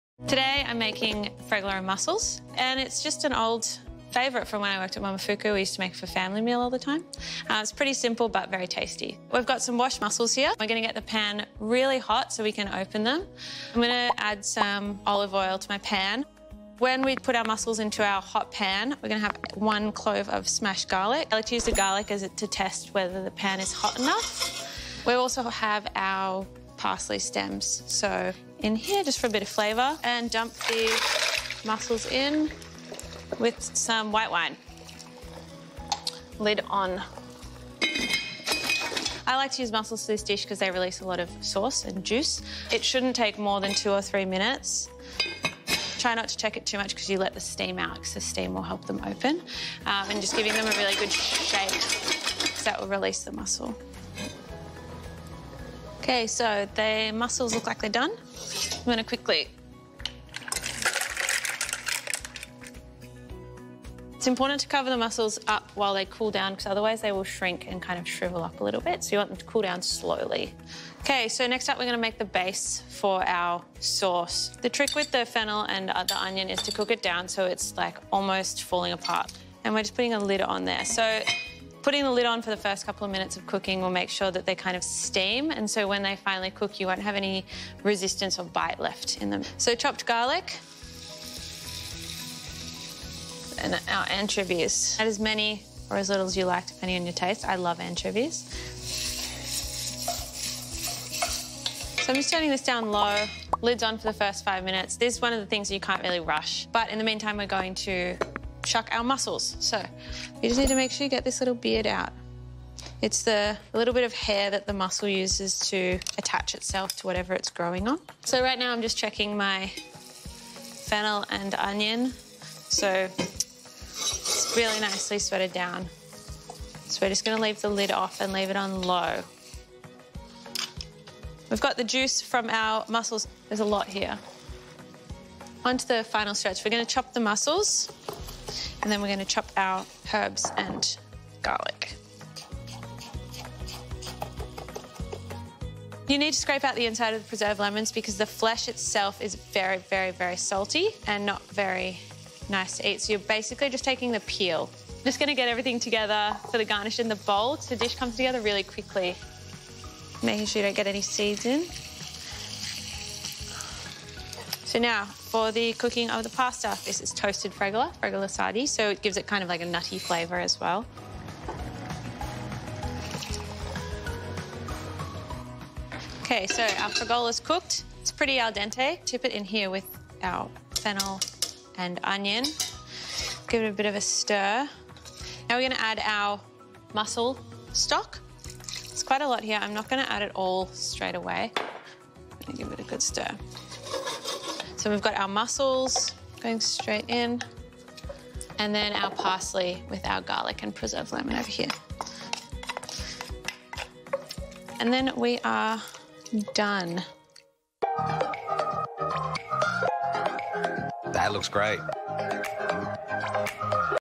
Fregola And Mussels seafood | Fregola Pasta with Vongole Clams
how to make easy & delicious recipe fregola and mussels
It is one of the best favorite family meals of Italian sea food, learn to prepare it in a short time to be lunch in restaurants and in family meals, it is a wonderful Italian dish and everyone loves it from fregola and mussels

The head chef at The Old Fitz in Sydney's Woolloomooloo shares an old restaurant family-meal favourite. Anna's tip: cool the mussels slowly so they don't shrink.

* INGREDIENTS
2kg mussels
4 tbsp olive oil
1 whole garlic clove, plus 5 cloves, minced
200ml white wine
2 onions, sliced
1 large bulb fennel, sliced
2 anchovies, or more to taste
500g toasted fregola
1 bunch parsley, leaves finely chopped, stems reserved
20g preserved lemon peel, finely chopped
zest and juice of 1 lemon
100g butter, cold, cubed
1 tbsp dried chilli flakes

METHOD

1- Wash mussels very well in cold water.

2- Place a large, lidded pot over high heat. Once hot, add the oil. Use the back of a knife to smash the whole garlic clove on a board. Add the smashed clove and the parsley stems to the oil.

3- Dump the mussels in and add a generous glug of white wine – you might need to do this in two batches depending on the size of your pot. Put the lid on, shaking every 30 seconds, until the mussels just start to open, about 2-3 minutes.

4- Tip the mussels out into a colander placed over a large bowl and cover with a tea towel to cool.

5- While the mussels are cooling, add another 2 tablespoons of olive oil to the same pot over low-medium heat and add the onions, fennel, anchovies, 4 cloves of minced garlic and a pinch of salt. Put the lid on and cook until everything becomes soft, about 5 minutes.

6- Remove the lid and continue to cook until the liquid has evaporated and onions and fennel become caramelised. Remove from heat.

7- Shuck mussels and remove beards. Roughly chop. Strain the reserved liquid into a small jug to remove any broken shells/beard.

8- Bring a large pot of salted water to the boil. Cook fregola for about 5 minutes, until al dente. Drain, and return to the pot with the onion and fennel.

9- Add the chopped mussels and reserved mussel liquid to the pot. Stir over low heat to warm everything through. Add the chopped parsley, the last minced clove of garlic, preserved lemon, and lemon zest and juice. Remove from heat, and stir through the cold butter. The mix should be a little soupy. Crack in a fair amount of black pepper and add the dried chilli. Serve with more chilli on the side for those who like it hot.

This recipe is from episode five of Good Food's new TV show, Good Food Kitchen,